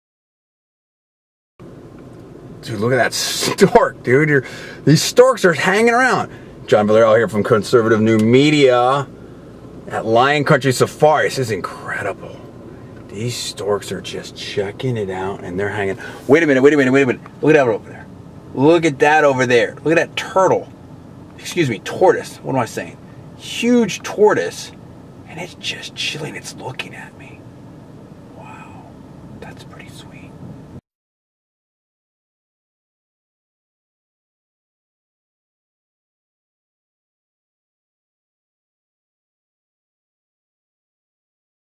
Storks and a Tortoise - by John D. Villarreal
JDV went to Lion Country Safari in Florida and saw some really cool animals.